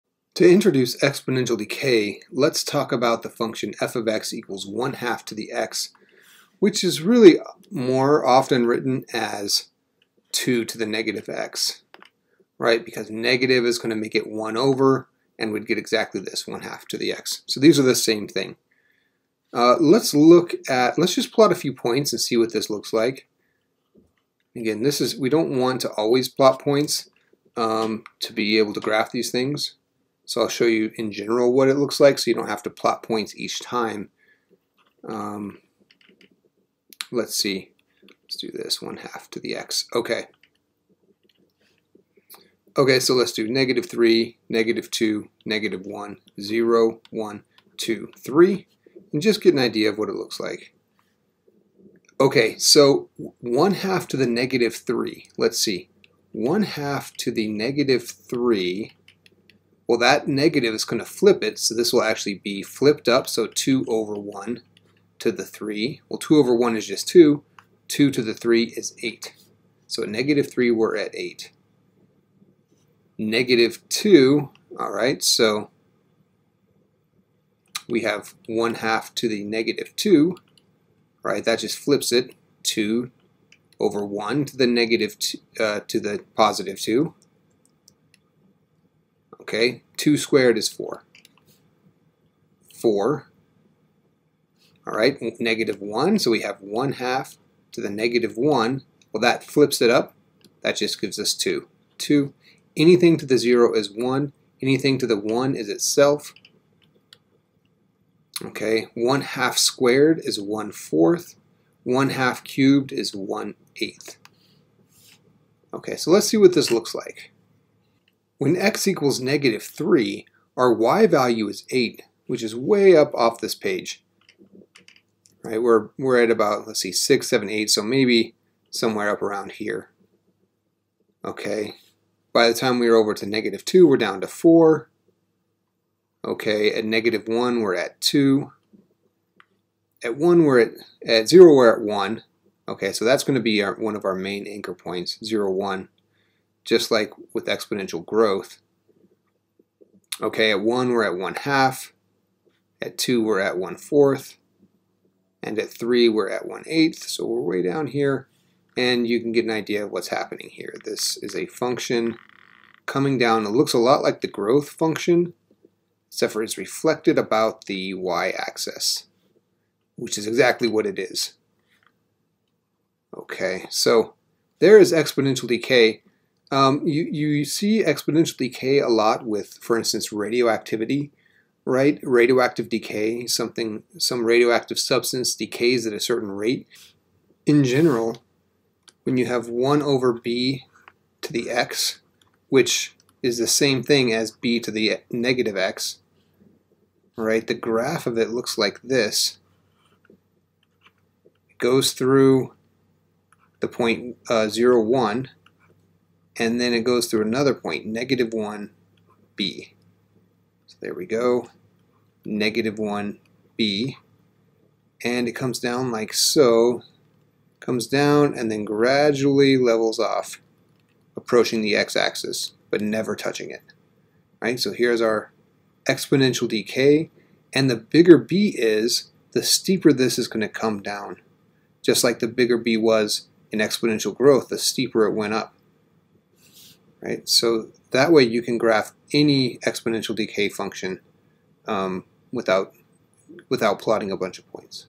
Introduction to Exponential Decay
This video introduces exponential decay with f(x)=(1/2)^x, which is the same function as 2^-x.  The method uses plotting points to graph the function and then introduces a general way to graph all exponential decay functions.